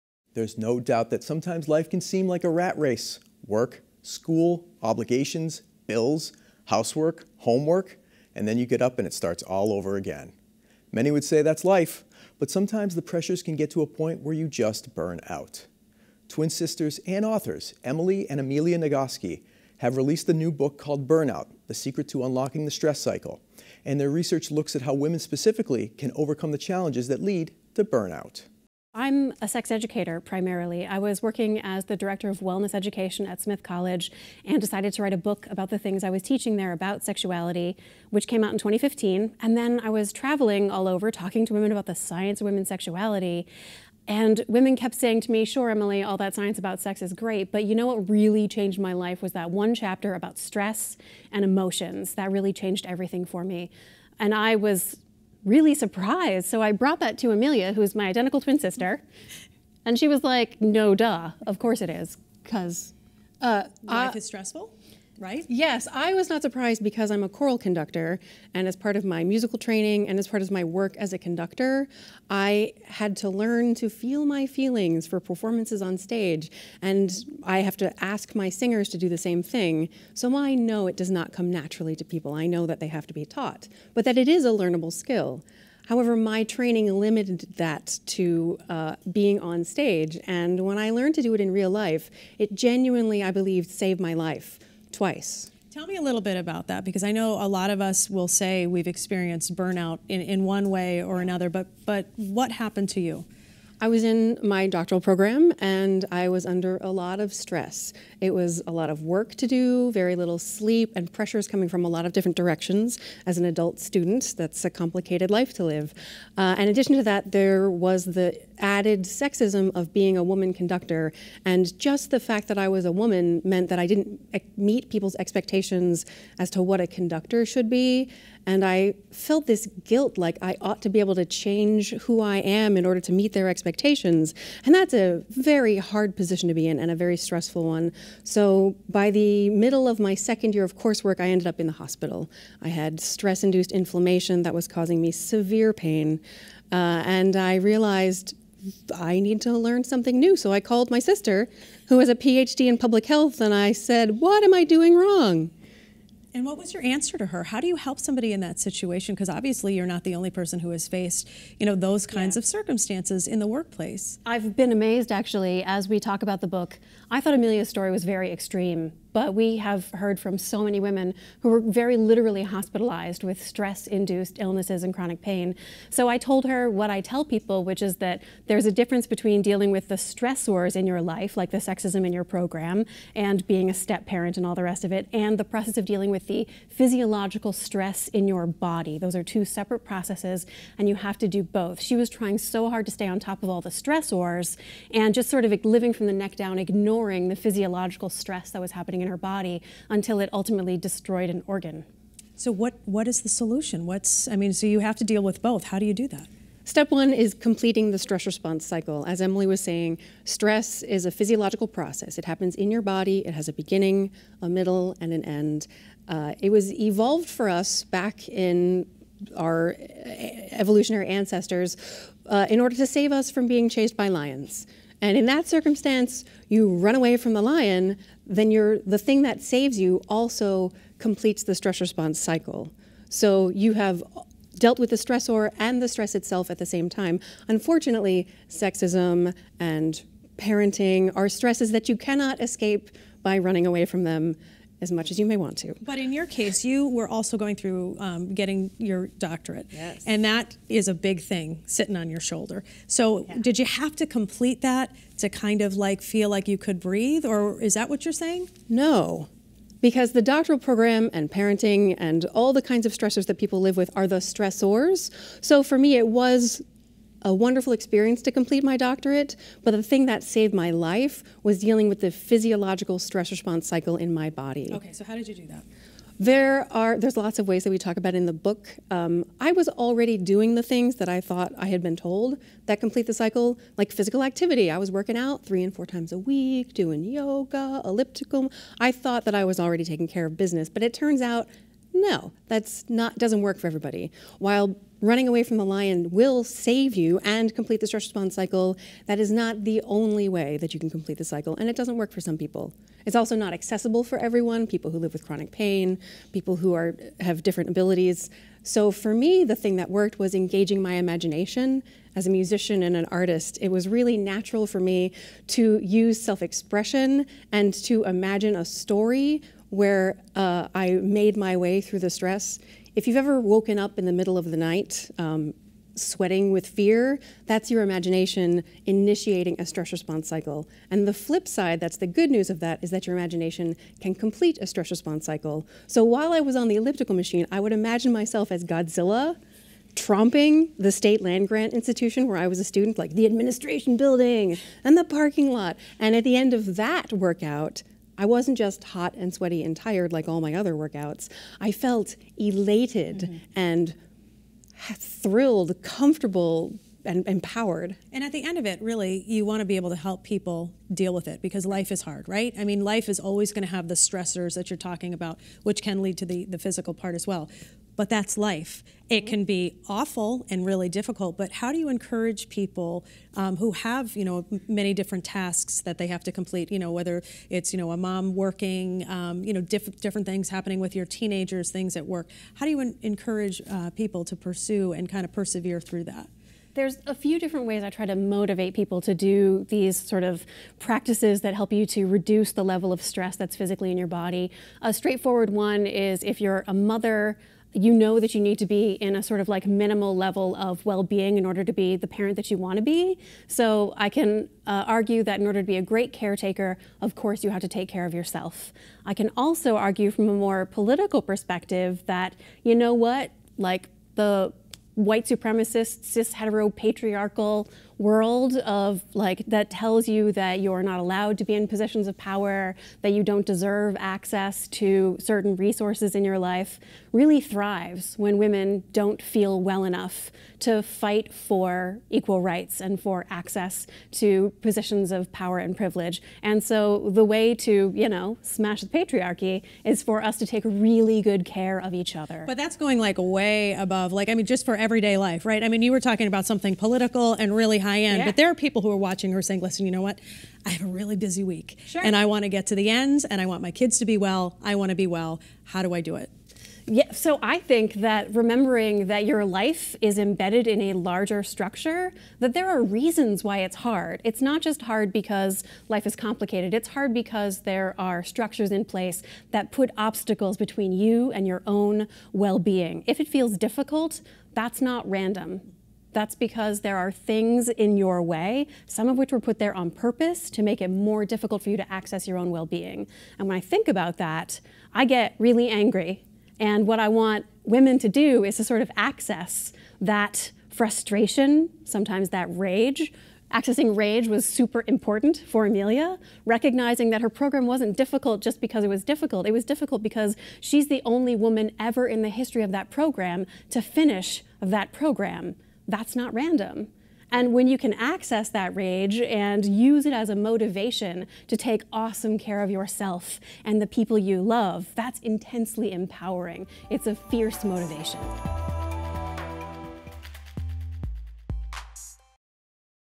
Overcoming Burnout Authors Emily & Amelia Nagoski | Connecting Point | April 9, 2019
Life can sometimes seem like a rat race: work, school, obligations, bills, housework, homework…and then you get up and it starts all over again. Many would say that’s life, but sometimes the pressures can get to a point where you experience burnout. Twin sisters and authors Emily and Amelia Nagoski new book “Burnout: The Secret to Unlocking the Stress Cycle” look at burnout and how women specifically can overcome the challenges that lead to burnout.